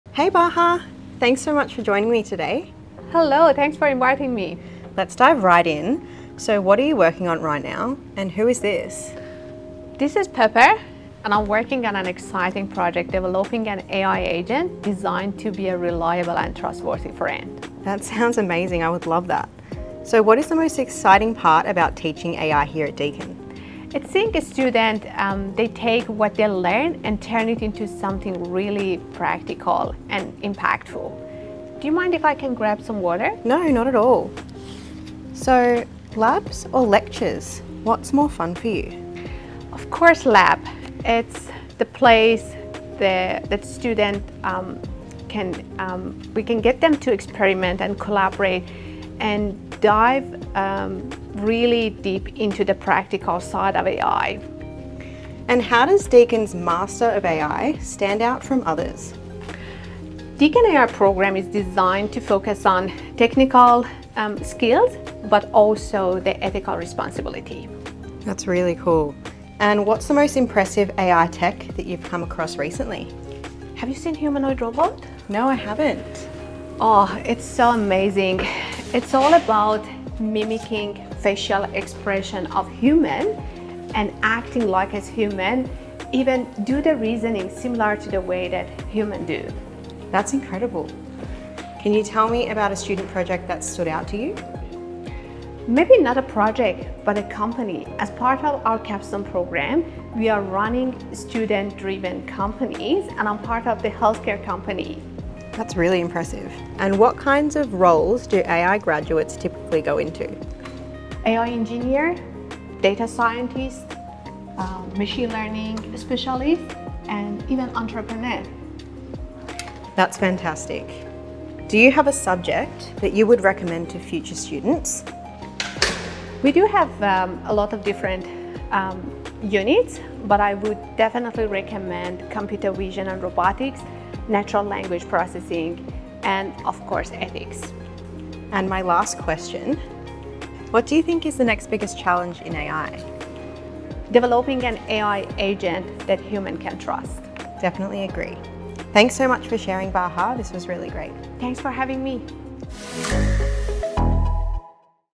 Meet the Master of Applied AI Course Director, Dr Bahareh Nakisa
Meet Dr Bahareh Nakisa, Course Director of Deakin's Master of Applied AI, as she shares insights on her favourite recent advancements in AI, must-study subjects for AI students, and her research developing trustworthy AI.

You can book a 1:1 consultation with Dr Nakisa and receive personalised advice on why Deakin is the right choice for your career aspirations